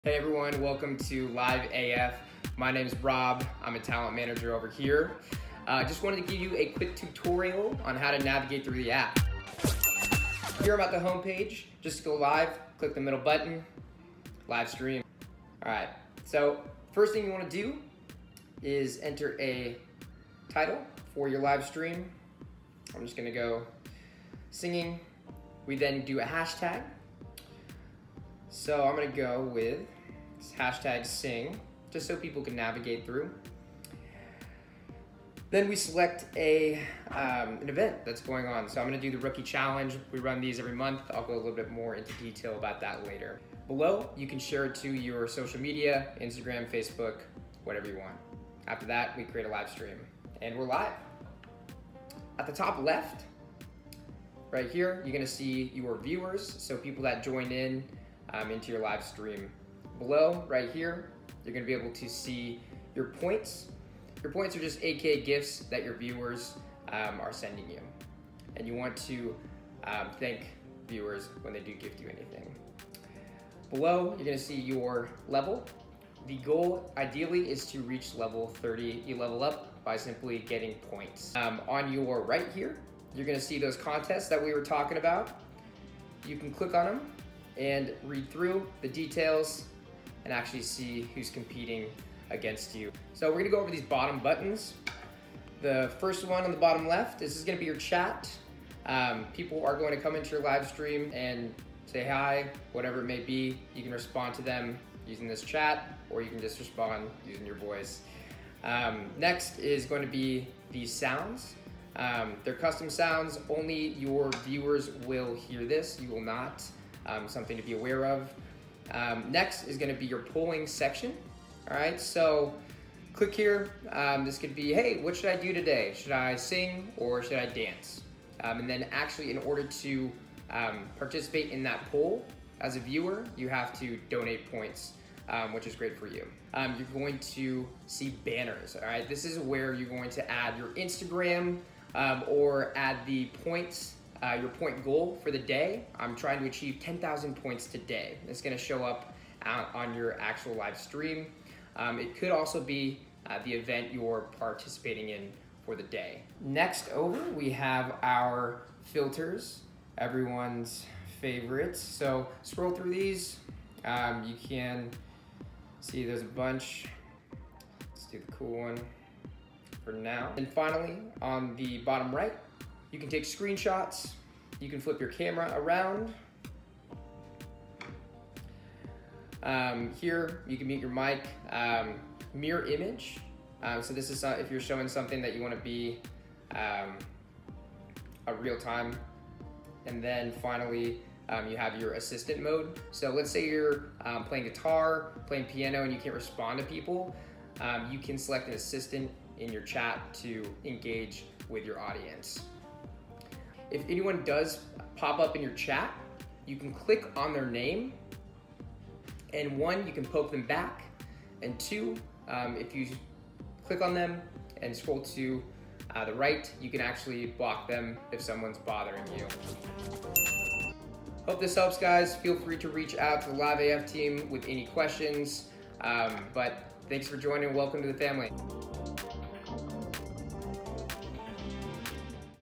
Navigating Through The App
This video will help you learn how to use our app as a streamer!